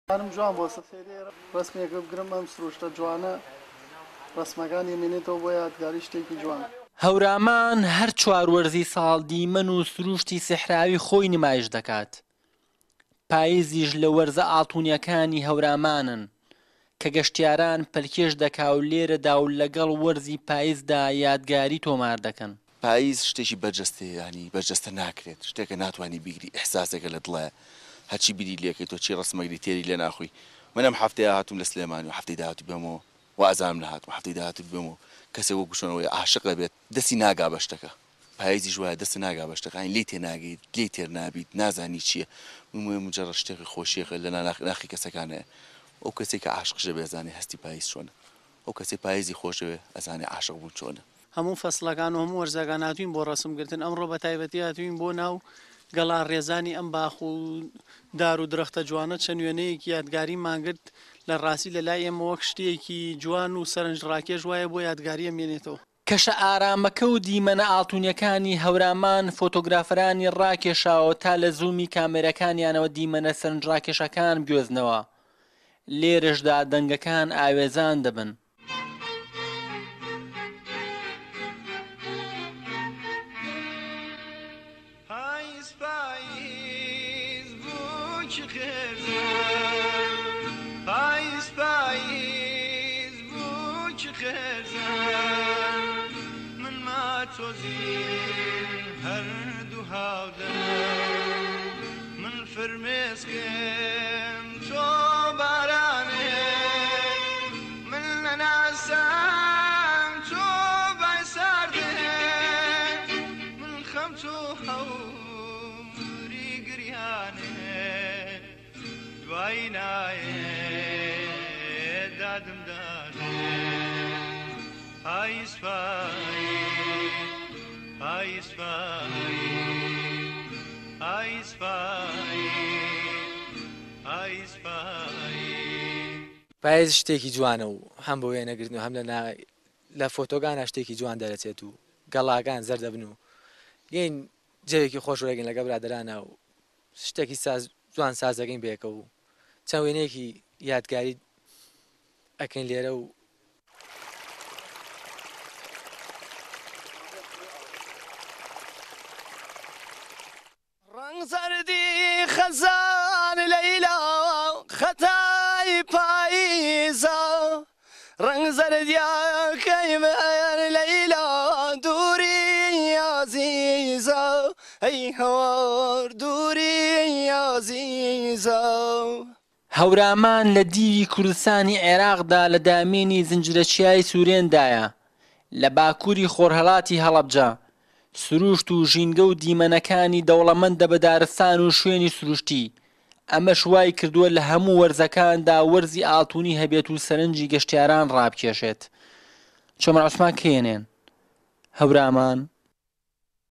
پایز ... وەرزە ئاڵتونیەكەی هەورامان
هەورامان هەر چوار وەرزی ساڵ بە دیمەن و سروشتی سیحراوی خۆی نمایش دەكات، پایز بە وەرزە ئاڵتونیەكەی هەورامان ناسراوە، كە گەشتیاران پەلكێشدەكات و لەگەڵ وەرزی پایزدا یادگاری تۆمار دەكەن.